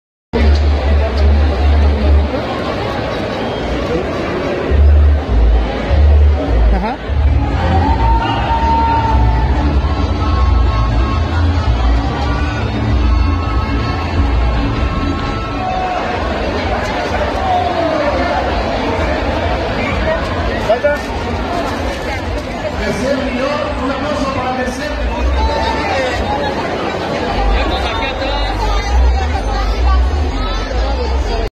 Decenas de personas asisten a la Feria del Taco de Canasta en San Vicente Xiloxochitla, Nativitas
Decenas de personas asisten a la Feria del Taco de Canasta en San Vicente Xiloxochitla, Nativitas, donde se repartieron miles de tacos de frijol, chicharrón y papa, como lo marca la tradición.